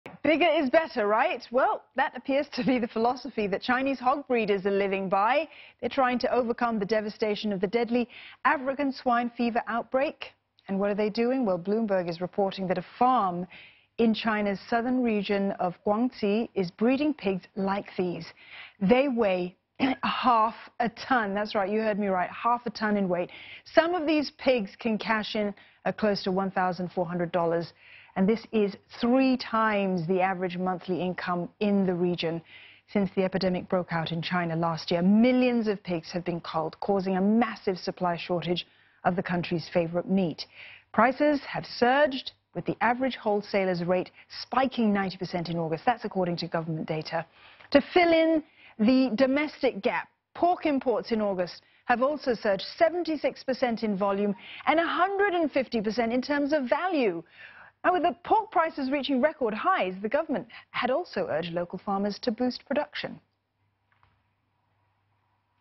Chinese farmers breeding giant pigs to combat pork shortages after swine fever
Chinese hog breeders are trying to overcome the devastation of the deadly African swine fever outbreak by breeding pigs that weigh half a tonne. Some of these pigs can cash in at close to $1,400. Since the epidemic broke out in China in 2018, millions of pigs have been culled, causing
a massive supply shortage of the country's favourite meat. Pork prices have reached record-highs.